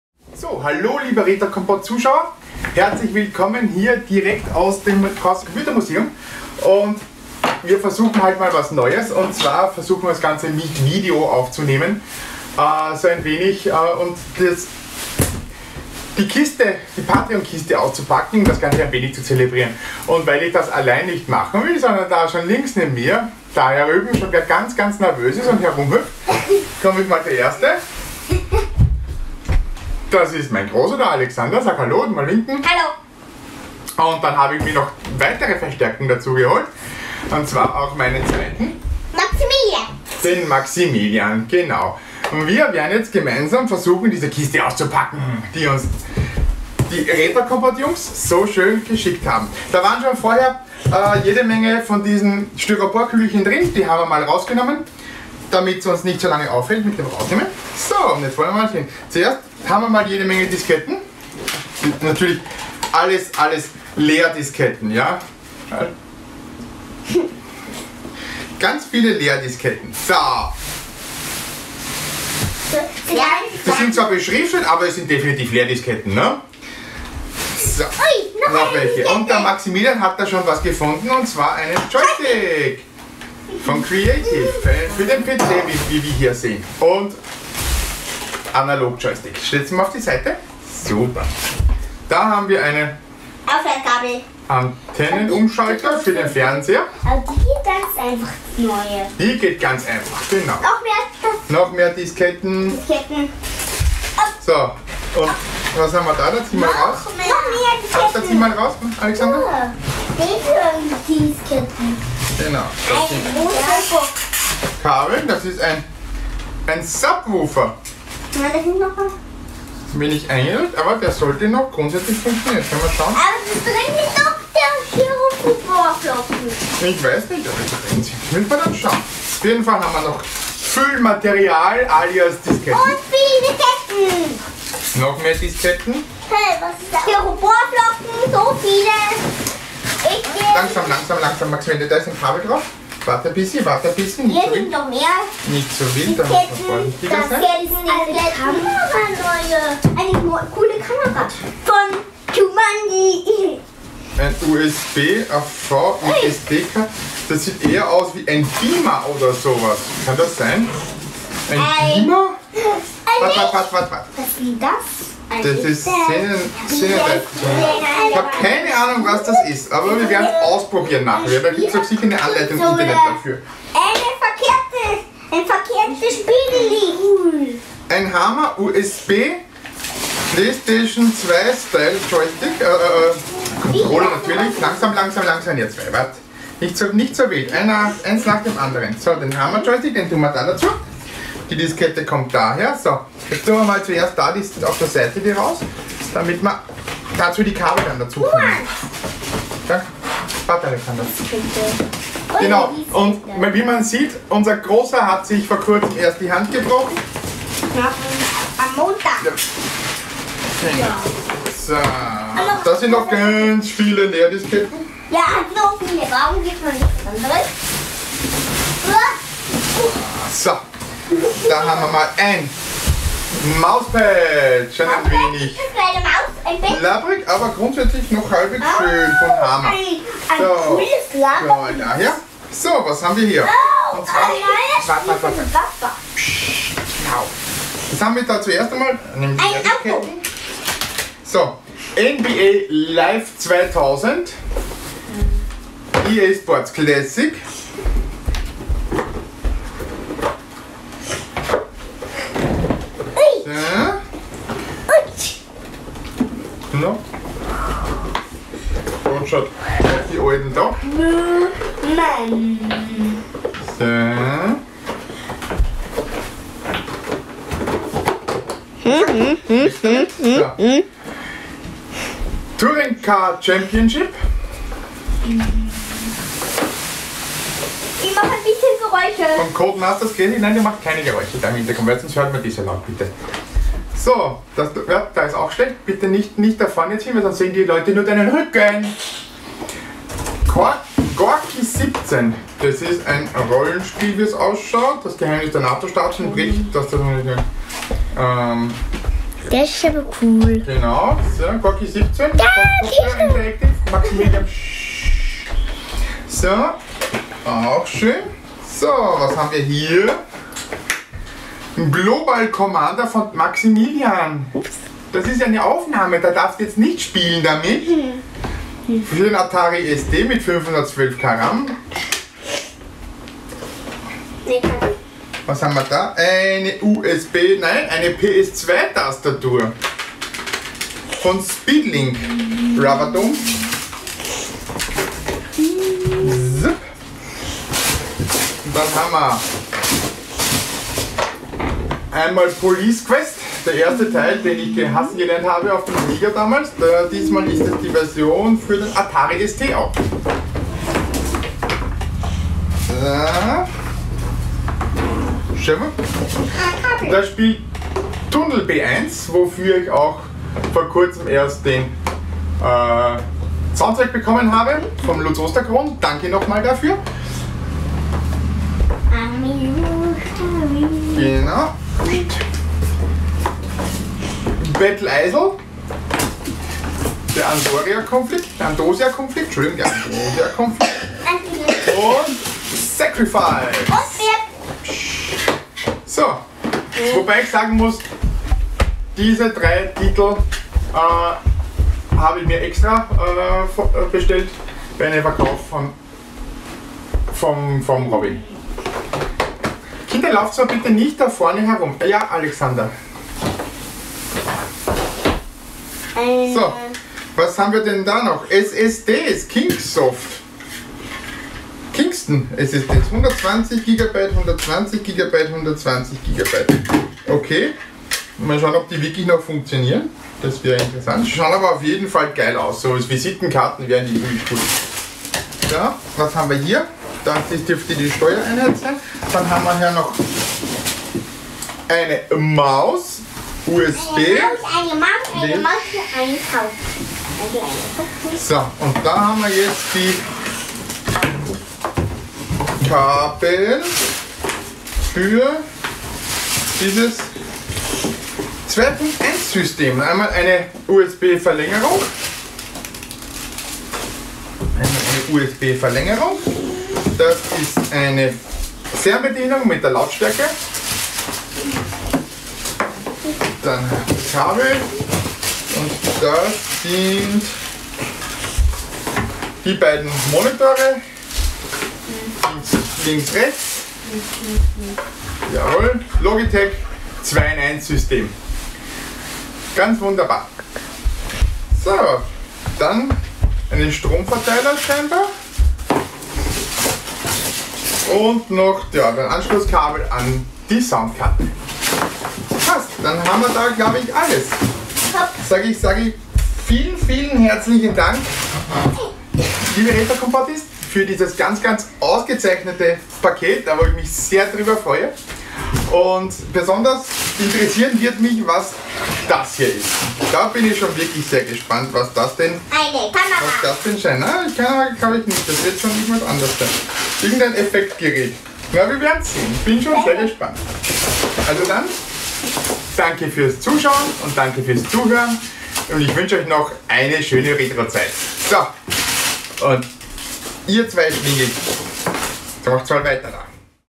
Auspacken Patreon Paket Juli 2021
unser Podcast Unterstützer Andreas packt sein Juli
Überraschungspaket aus.

Retrokompott Unterstützer auf Patreon oder Steady
erhalten bei uns exklusive Podcasts und vieles mehr.